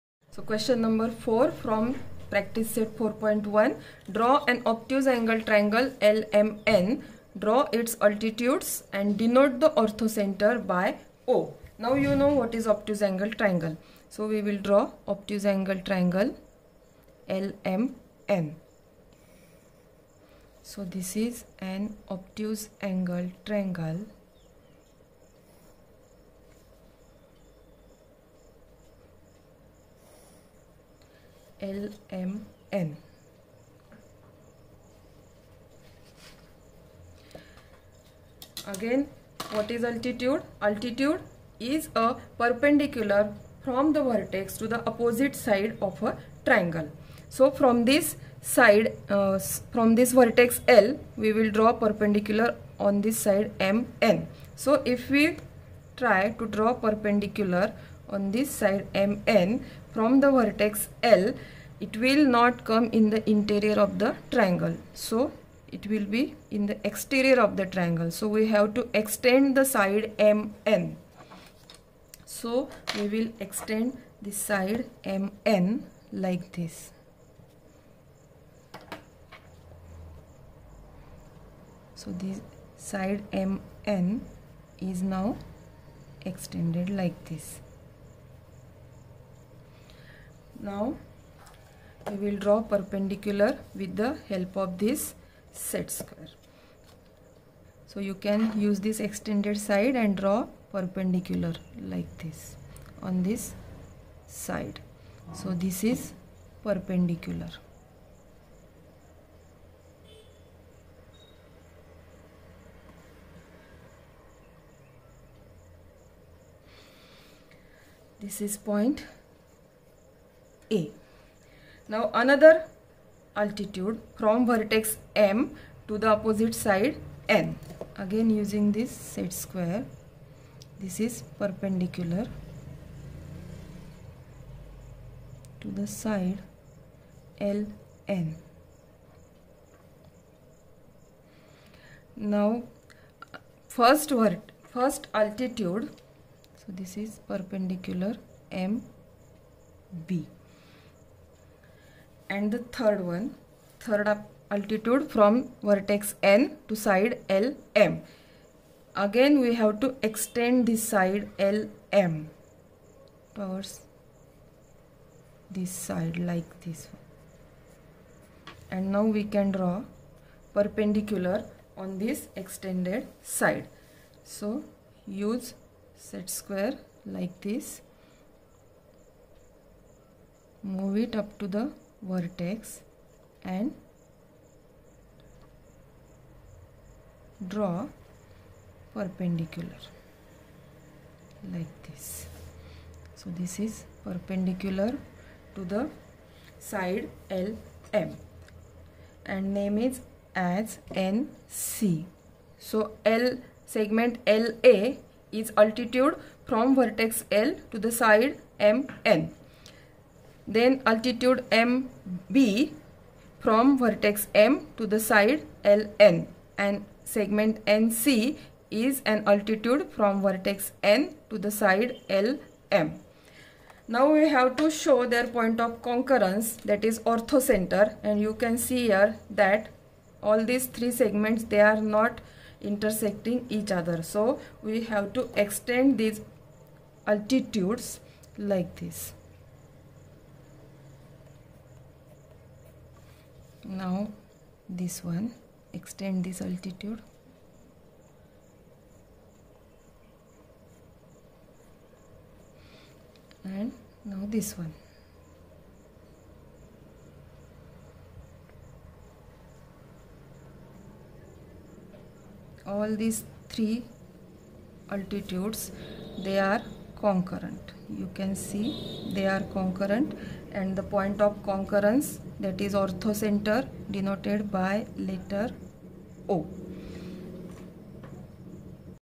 Altitudes in Obtuse Angled Triangle | Denoting Orthocentre.
8th MATHS

Chapter 4th : Altitudes and Medians of a Triangle.

Practice set : 4.1 - Que4